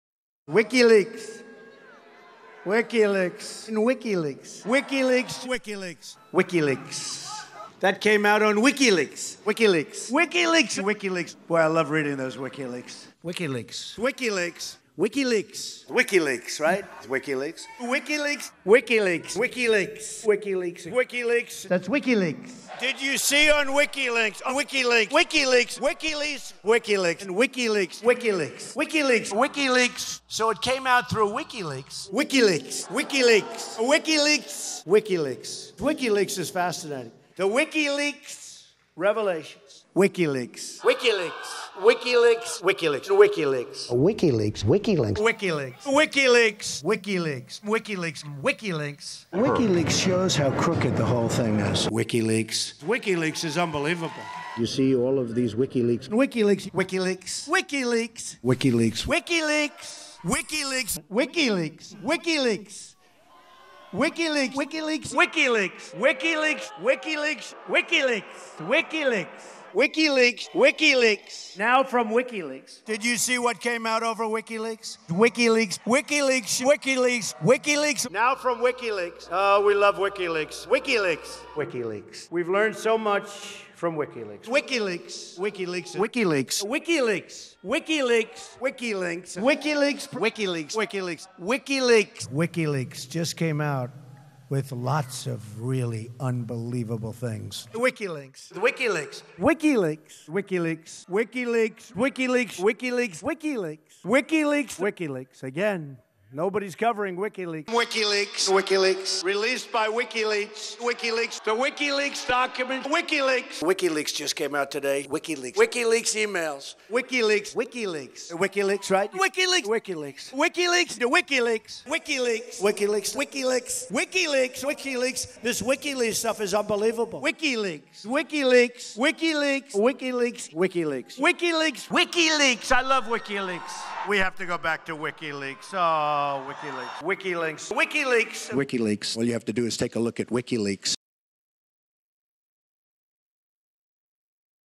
Trump says WikiLeaks 141 times in month before election
(NBC Mix for archives)